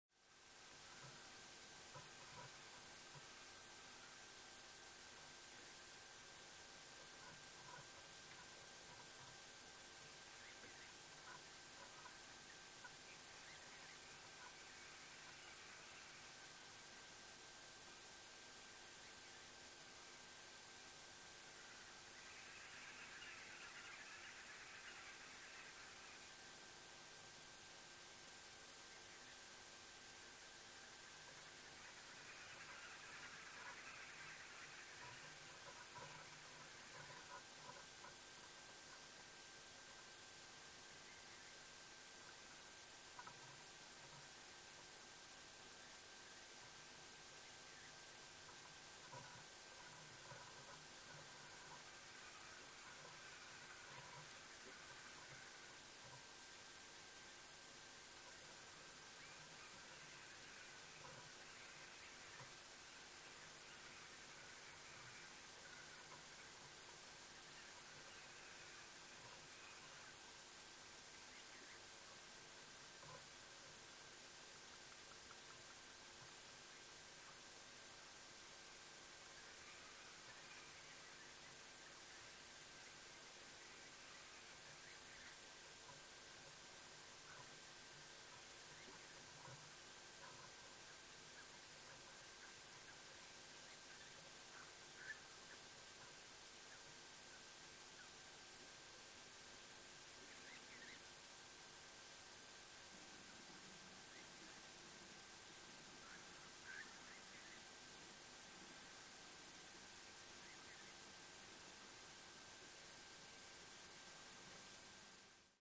A Beautiful African Fish Eagle
An African fish eagle perched on a dead branch